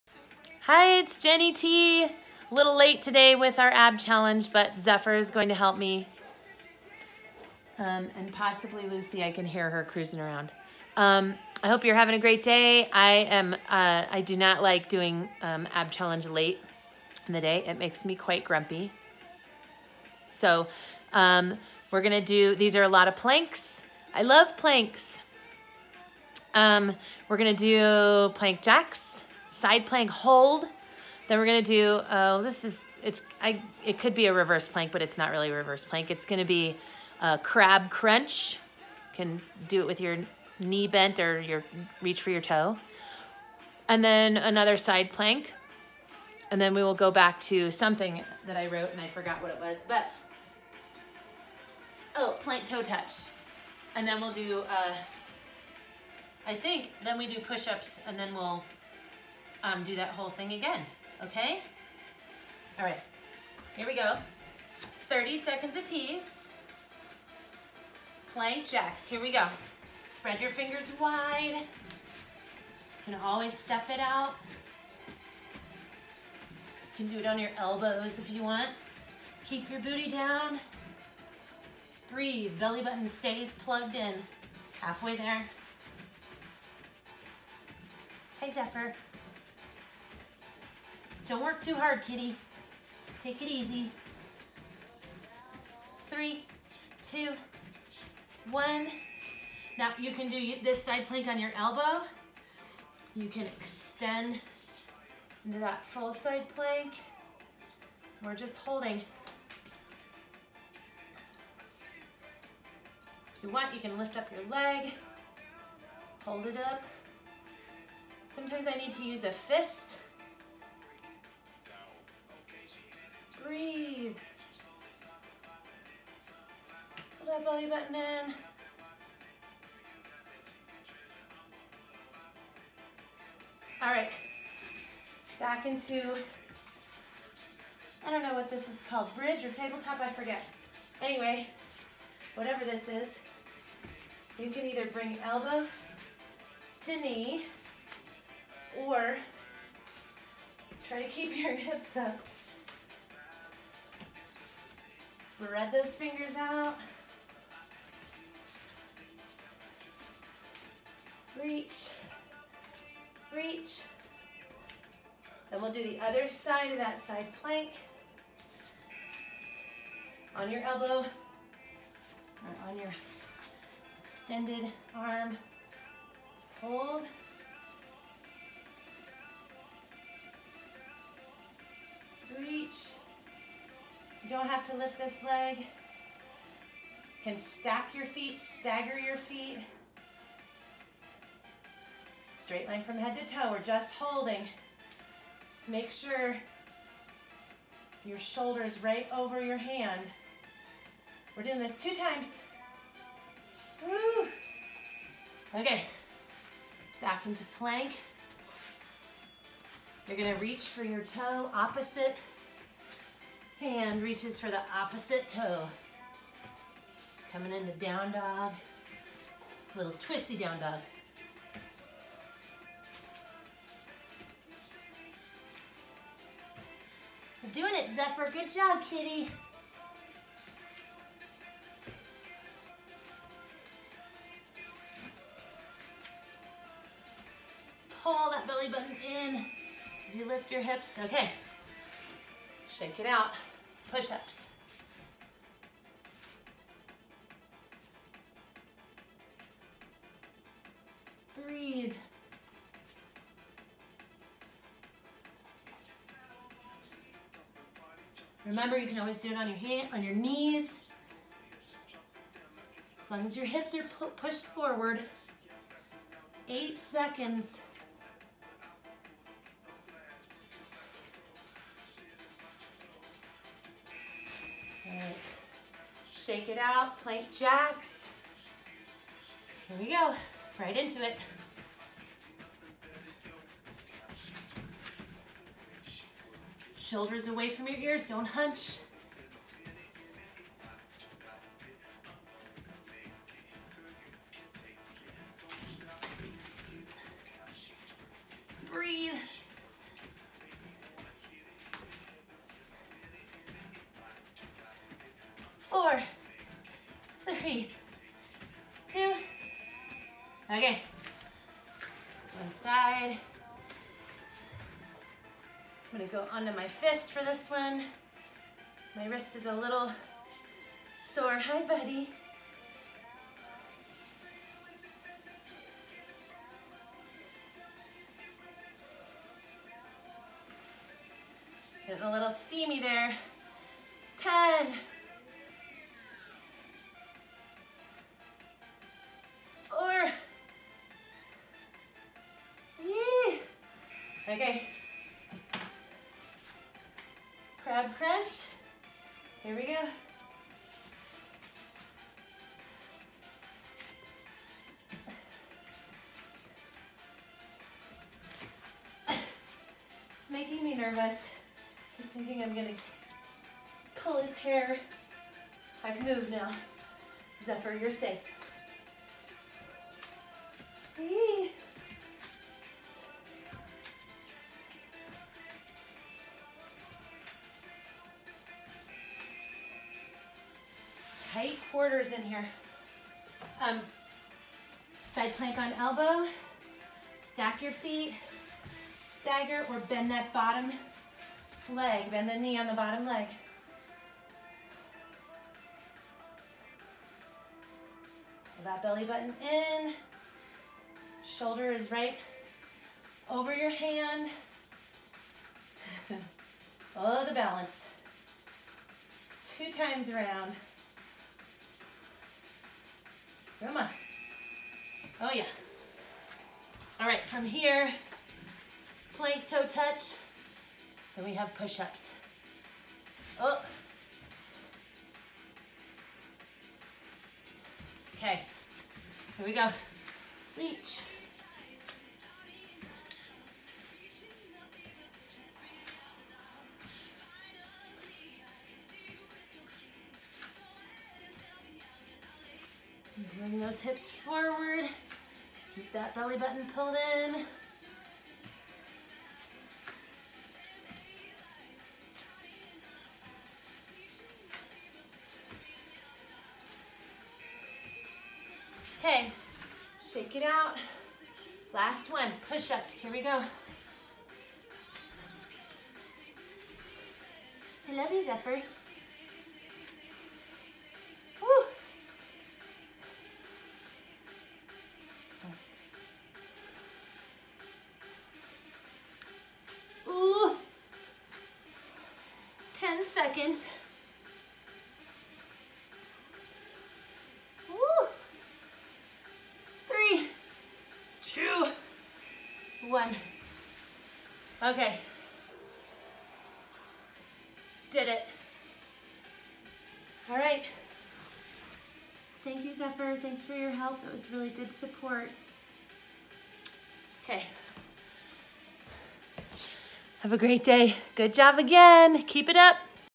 Late in the Day Ab Challenge
Well, better late than never, as they say. Here It is!

:30 each, 2 rounds of the same 5

-Plank Jacks
-Side Plank Hold
-Crab Crunch
-Other Side Plank Hold
-Push ups

Repeat!

Yay! Have a wonderful day!